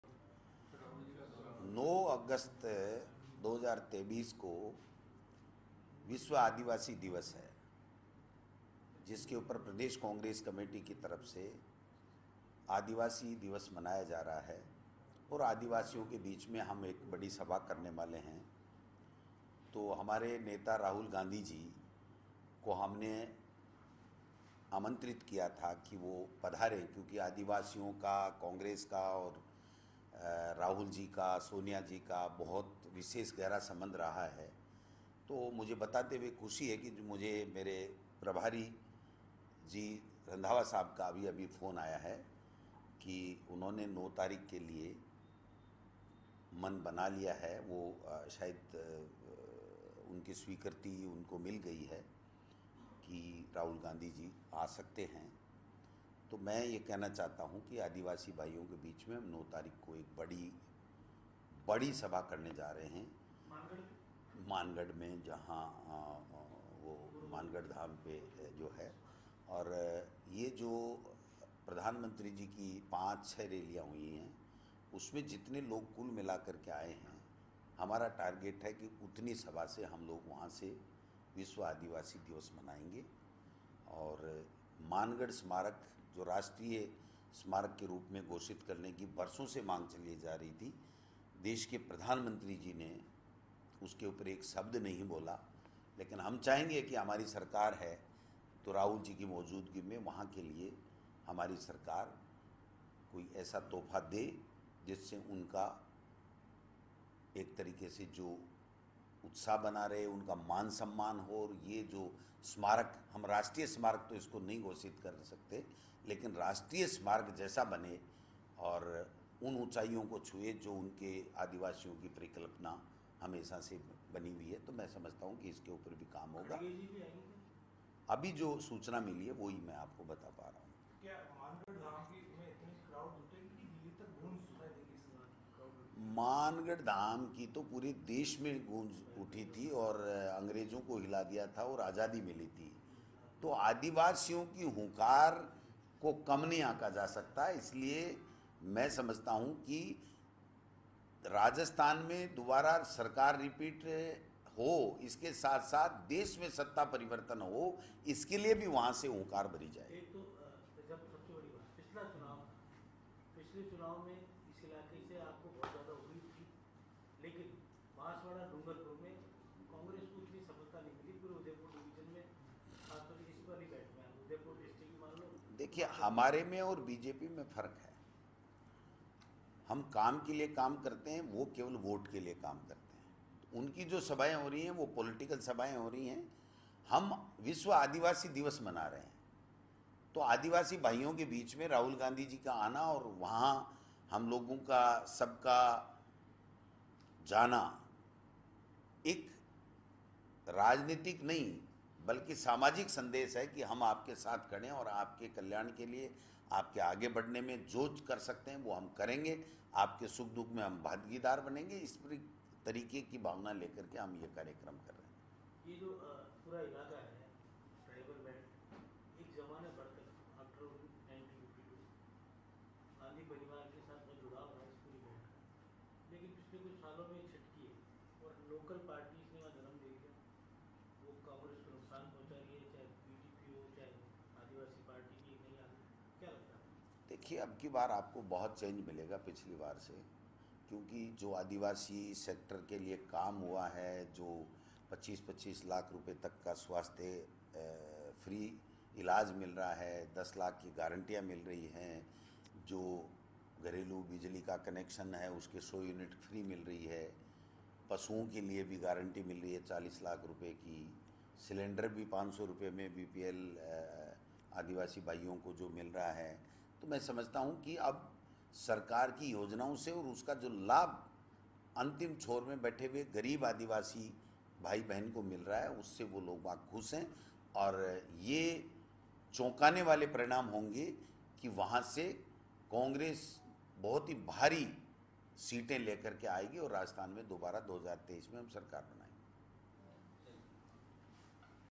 Rahul Gandhi 9 अगस्त को आएंगे मानगढ़ स्मारक: Govind Singh Dotasra | Rajasthan Politics
About Video:

'सीकर सभा में नहीं था जोश और न थी भीड़','हमारी गारंटियों का पहले मखौल उड़ाया', 'अब अपनी घोषणाओं को गारंटी बता रहे', 'जबकि राजस्थान को तो कोई गारंटी नहीं देकर गए', 'यहां के लोगों को ERCP की गारंटी चाहिए थी', 'जातिगत जनगणना की गारंटी चाहिए थी', 'महंगाई कम करने की गारंटी चाहिए थी', 'लोगों को गारंटी चाहिए थी', '2014 में उठाए मुद्दों पर गारंटी चाहिए थी', 'राजस्थान के लोगों को कोई गारंटी नहीं देकर गए', 'ऐसे में यहां की जनता क्यों वोट दे बीजेपी को ?', 'चुनावी जनसभा का कोई असर नहीं हुआ', 'उनके डबल इंजन की सरकार के दावे खोखले है

'राहुल गांधी के दौरे को लेकर तैयारी है', 'अभी तक राहुल गांधी ने कार्यक्रम तय नहीं किया', 'मानगढ़ के दौरे को लेकर हमने आदिवासी दिवस पर निवेदन किया है', 'राहुल गांधी आएंगे तो अच्छा मैसेज जायेगा', 'पीएम आए थे लेकिन आदियावसियों के हितों की बात नहीं की'

'राहुल गांधी का दौरा फाइनल', '9 अगस्त को आएंगे राहुल गांधी'

'पेपर लीक राष्ट्रीय मुद्दा है', 'एमपी में तो व्यापम घोटाला हुआ था', 'राजस्थान में हमने अन्य राज्यों से अधिक नौकरी दी', 'रोजगार को लेकर तो बीजेपी को श्वेत पत्र जारी करना चाहिए', 'पीएम कल नाराज होकर गए' , 'स्थानीय बीजेपी से नाराज थे', 'पिछली बार हारी 57 सीटों पर विशेष फोकस रहेगा', 'एक एक सीट पर सचिव को भेजा जाएगा', 'कोशिश रहेगी इन सीटों पर मिले विजय '

'मानगढ़ स्मारक पर आएंगे राहुल गांधी', 'विश्व आदिवासी दिवस मना रहे है', 'उसे मनाने के लिए राहुल गांधी आएंगे', '9 अगस्त को आएंगे राहुल गांधी' | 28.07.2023